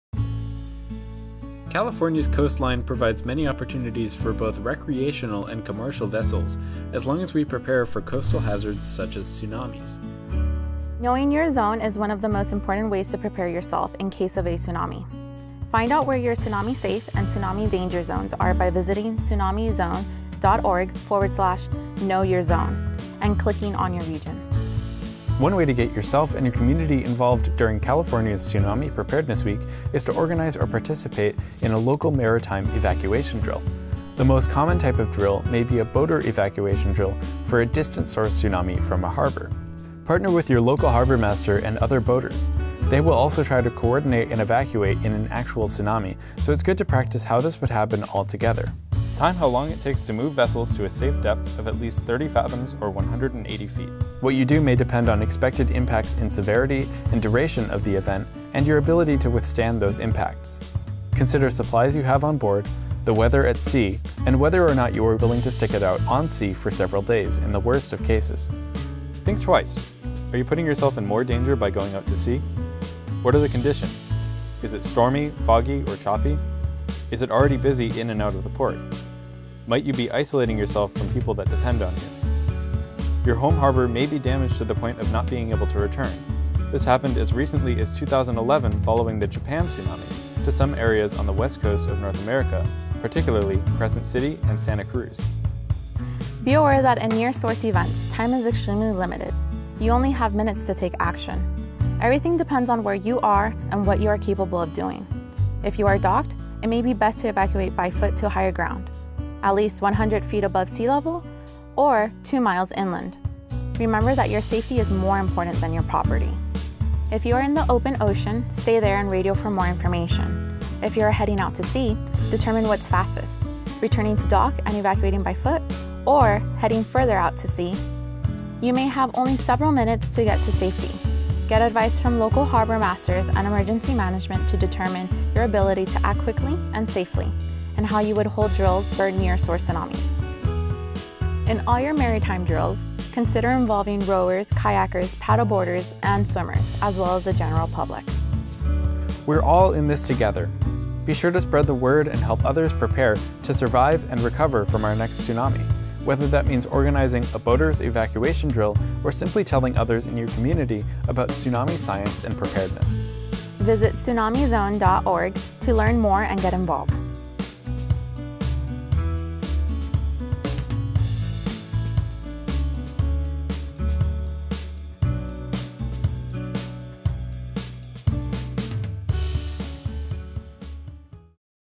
Maritime Tsunami Preparedness
Watch this video to learn about how to be prepared for tsunamis if you are at a port or operate a seafaring vessel, and visit TsunamiZone.org to know your zone, learn more, and register your own tsunami preparedness activities!